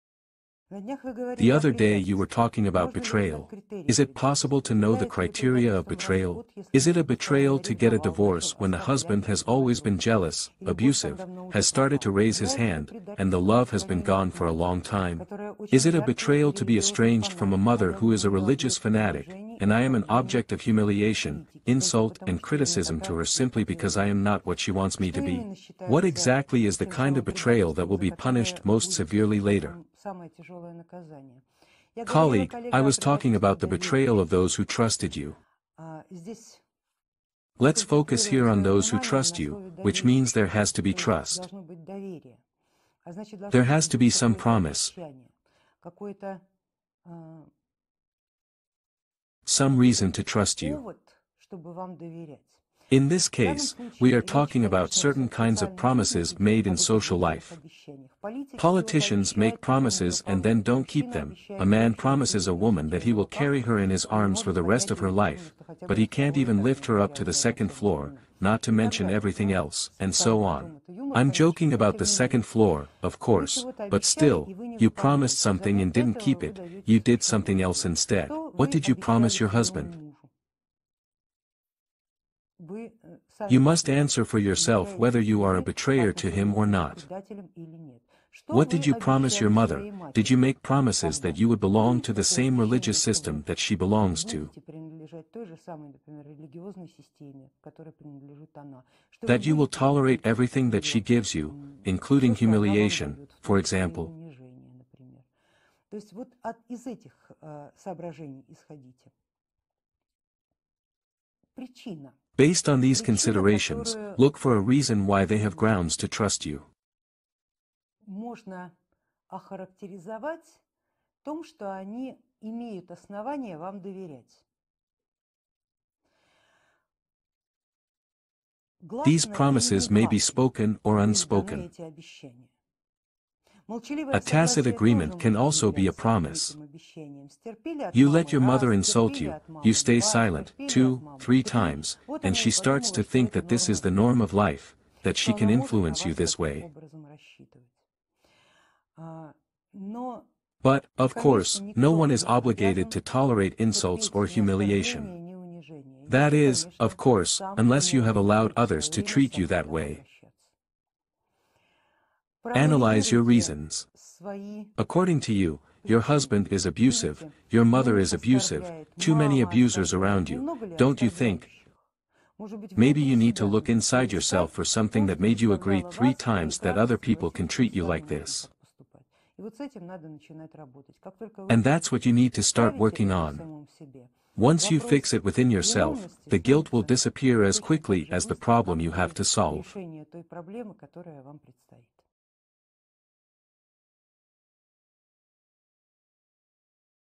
About Betrayal In Ordinary Life
About Betrayal In Ordinary Life. The other day you were talking about betrayal. Is it possible to know the criteria of betrayal? Is it a betrayal  to get a divorce when the husband has always been jealous, abusive, has started to raise his hand, and the love has been gone for a long time?
Is it a betrayal to be estranged from a mother who is a religious fanatic, and I am an object of humiliation, insult and criticism to her simply because I am not what she wants me to be?
- Colleague, I was talking about the betrayal of those who trusted you. Let's focus here on those who trust you, which means there has to be trust, there has to be some promise, some reason to trust you.
You promised something and didn't keep it, you did something else instead. What did you promise your husband? What did you promise your mother, did you make promises that you would belong to the same  religious system that she belongs to, that you will tolerate everything that she gives you, including humiliation, for example?